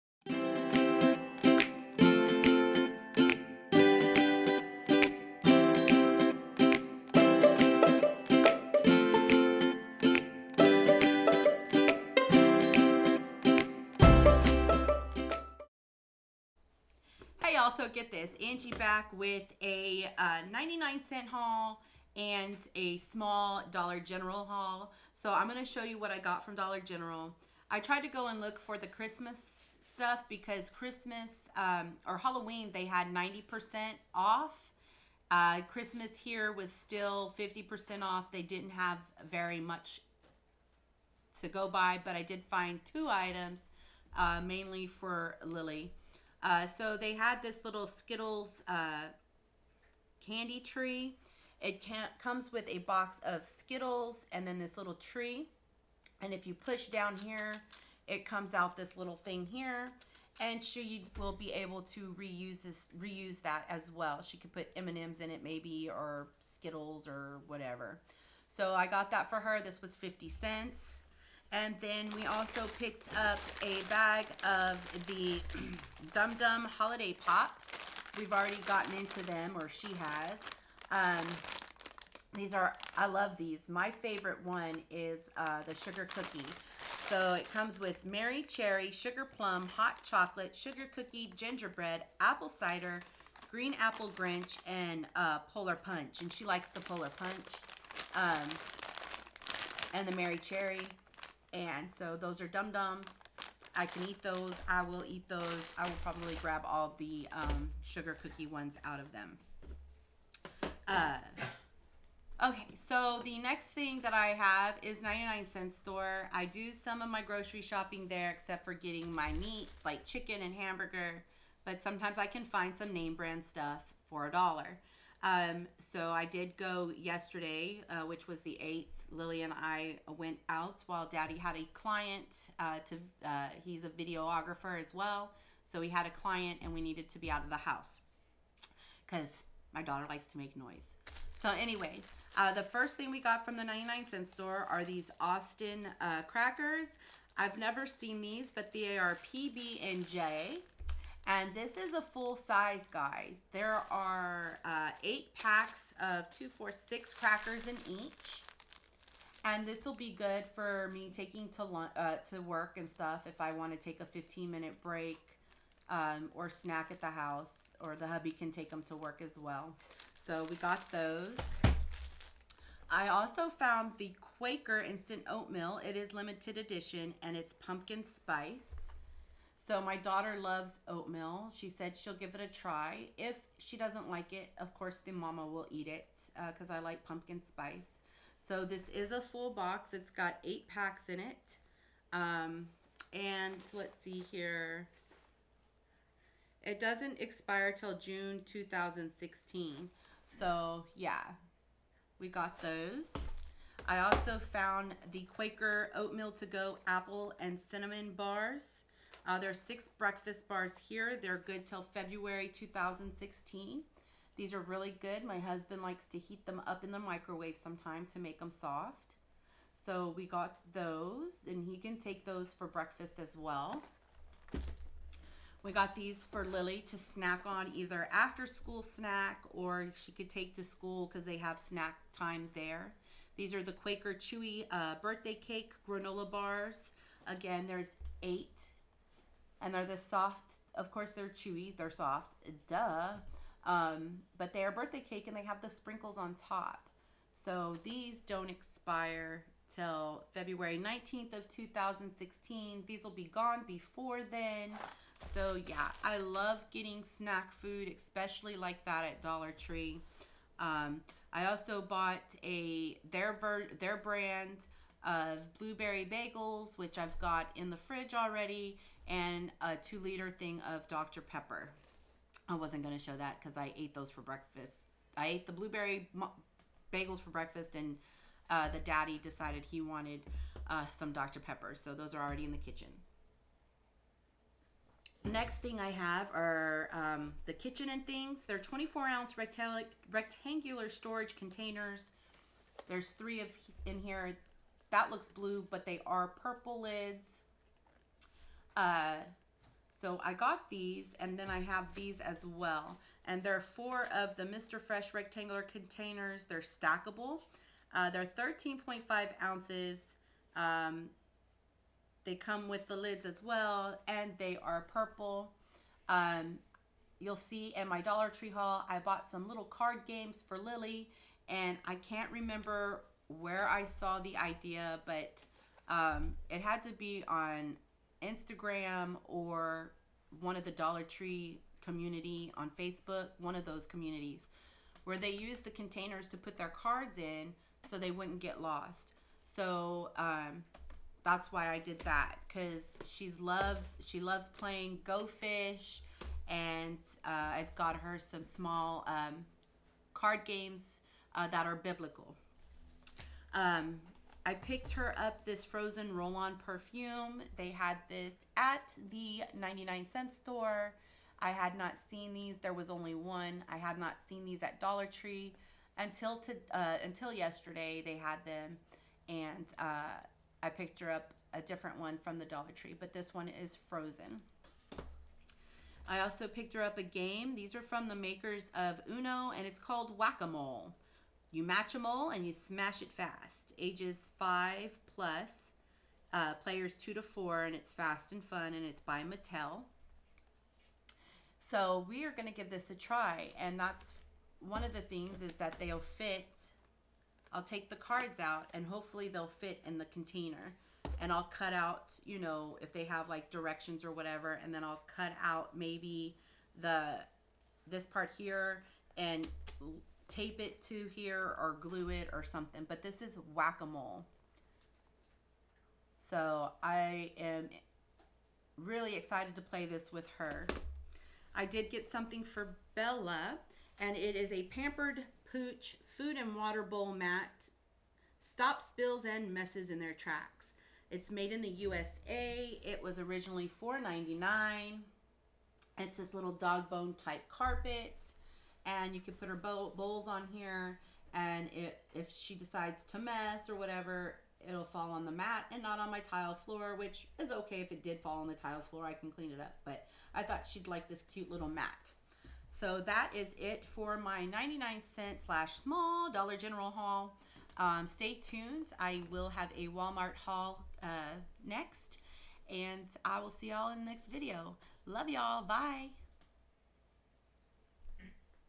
.99 Cents Only Store Haul W/ A Little DG!!
Hi ya'll!! I have a small .99 Cent Only Store haul with 2 items from DG for your enjoyment!! Love ya'll!! :0)